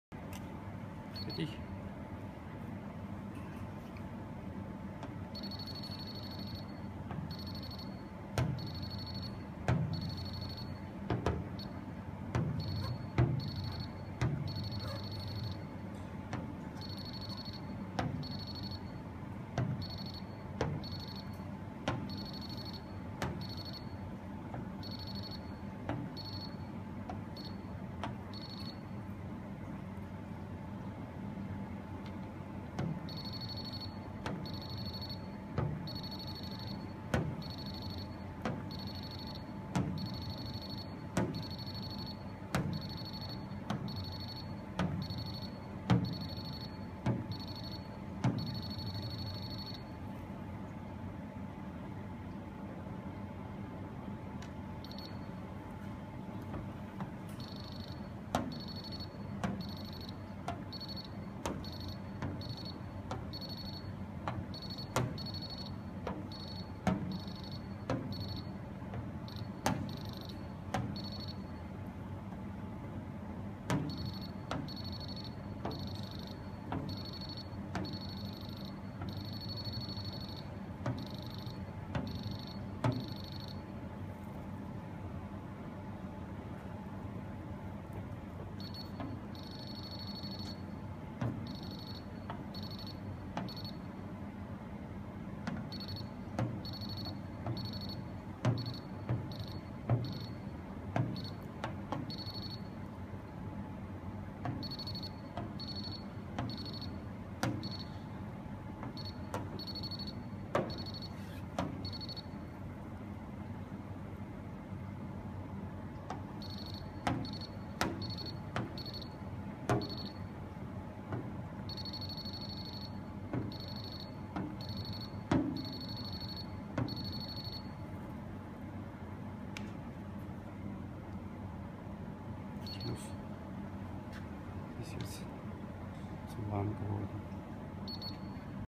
Froschbeseitigungsversuch auf der Strebe ohne sie abzuflexen
Hier wird gezeigt, dass auf der Strebe und auf den geschweißten Stellen nicht immer viel erreicht werden kann, aber trotzdem einiges möglich ist.